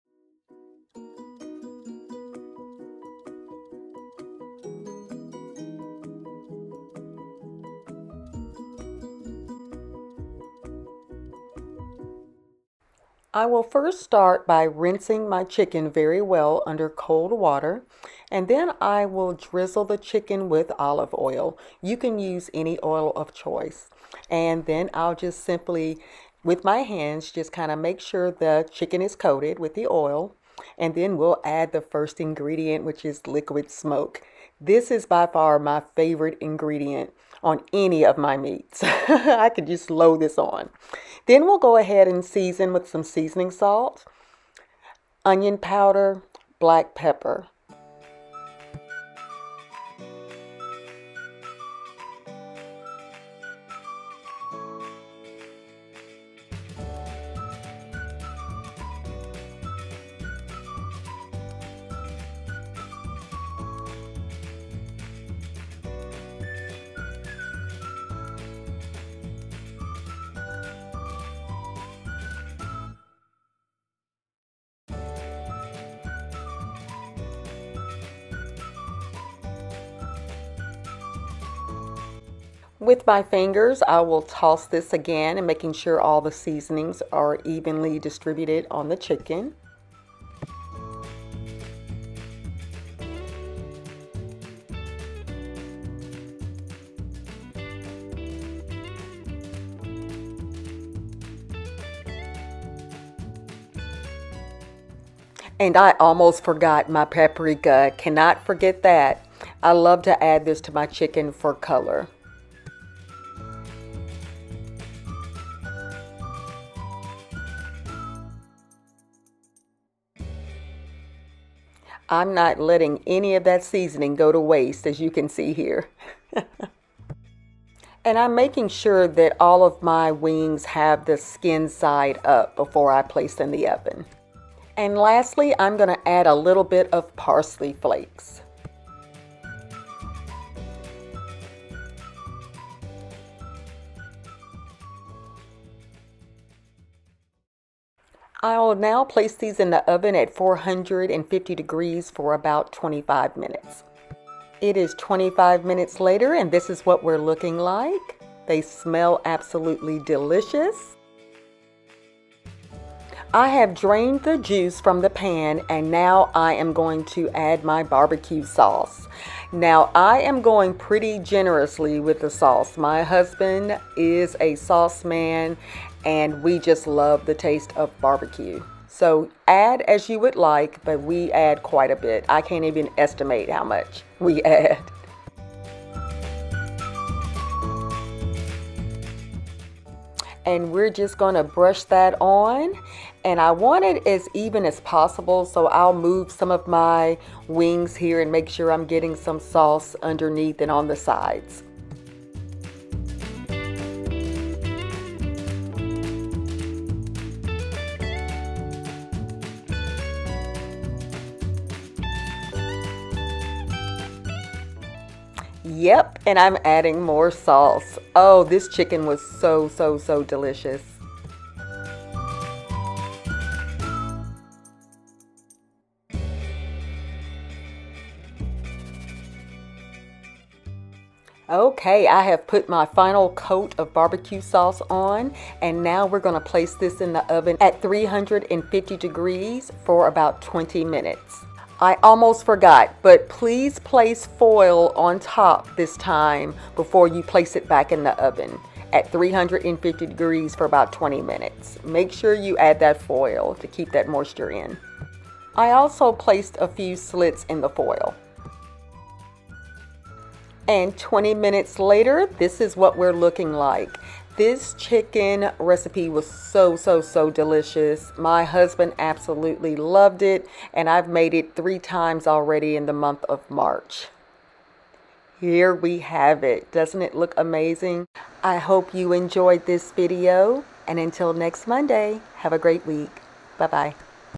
Easy baked bbq chicken wings
This is an easy baked BBQ chicken wings recipe using liquid smoke!

INGREDIENTS I USED:
Chicken wings
Olive oil
Seasoning Salt
Garlic Powder
Onion Power
Paprika
Liquid Smoke
Parsley Flakes
Sweet Baby Ray's Barbecue Sauce

Blessings & Love,

T